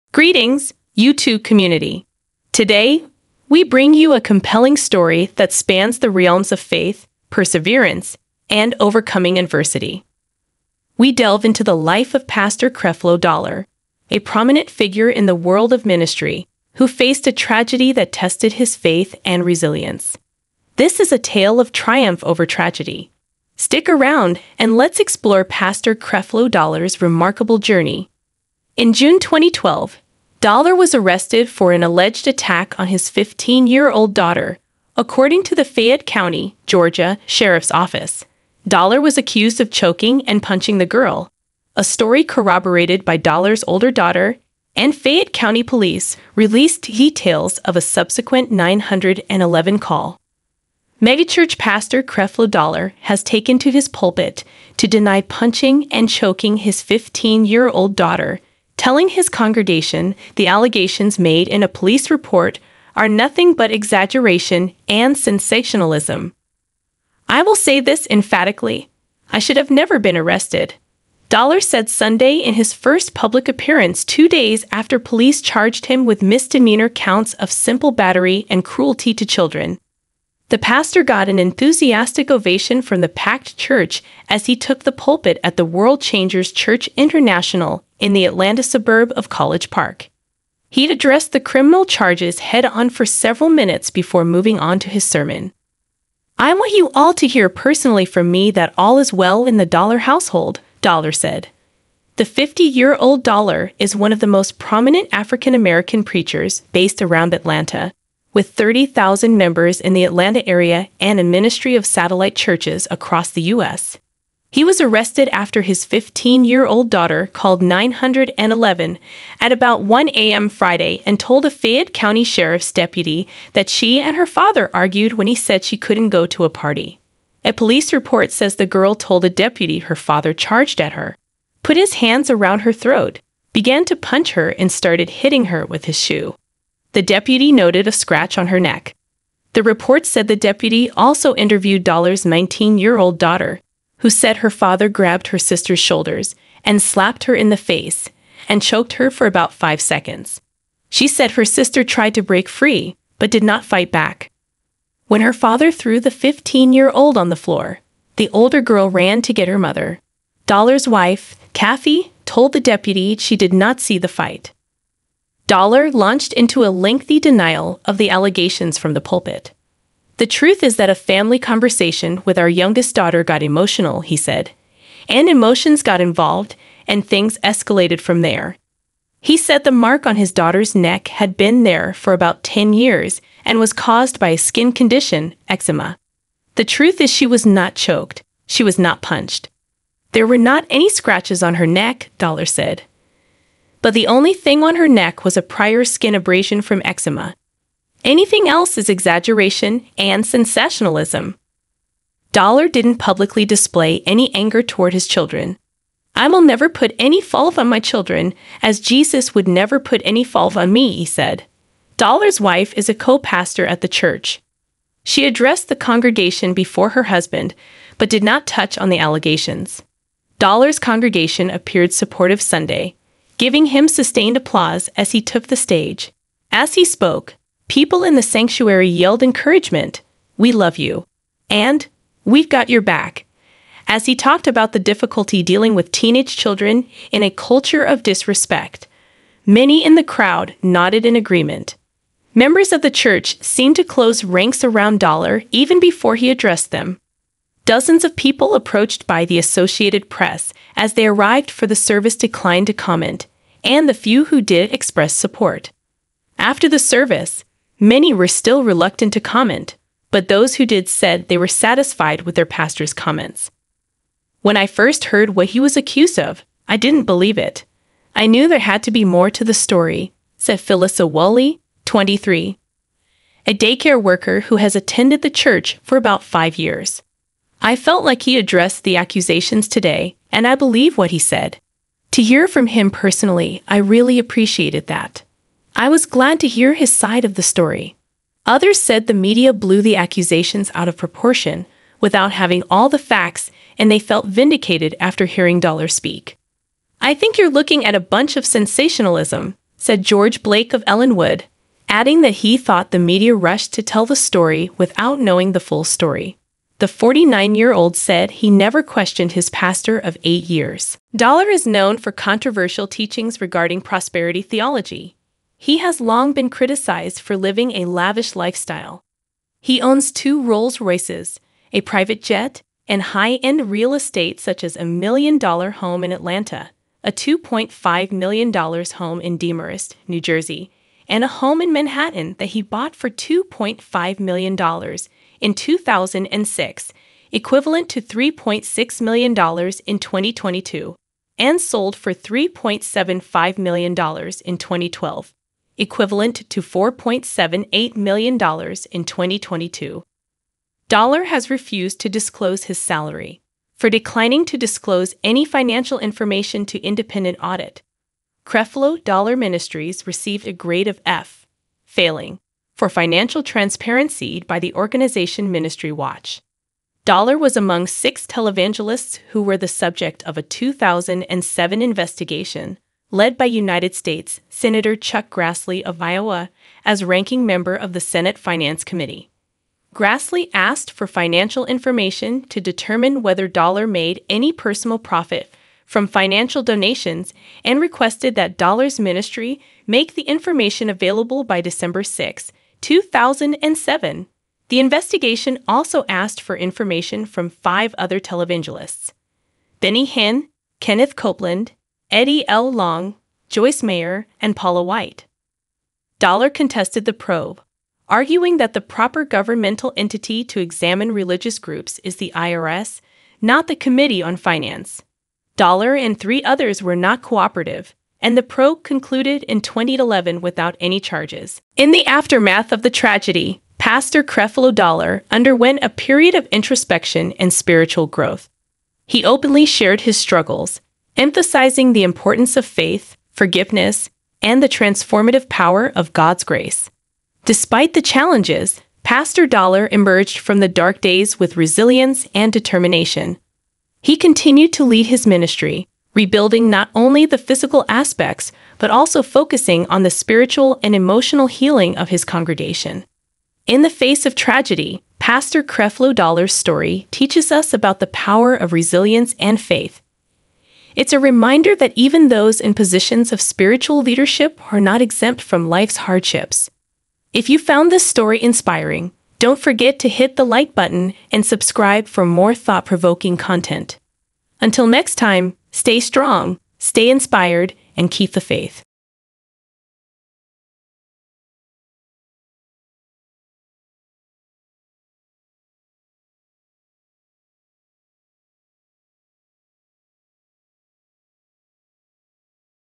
The MESSY Life of Pastor Creflo Dollar & Ending You Never Knew
Creflo Augustus Dollar, Jr., (born January 28, 1962) is an American pastor, televangelist, and the founder of the non-denominational Christian World Changers Church International based in College Park, Georgia, a suburb of Atlanta. Dollar also heads the Creflo Dollar Ministerial Association (formerly called International Covenant Ministries), Creflo Dollar Ministries, and Arrow Records.